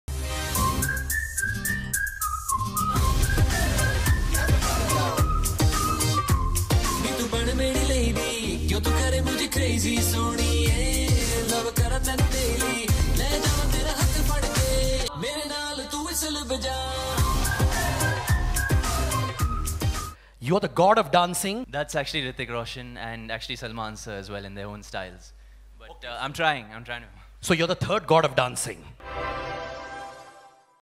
Tiger Shroff’s Awesome Dance Moves at IIFA Promotion Event
Tiger Shroff shows off his dancing moves at the IIFA Awards promotional event. It's just a glimpse of his performance at the upcoming award function. But told the gathering that Salman and Hrithik are god of dancing in their own style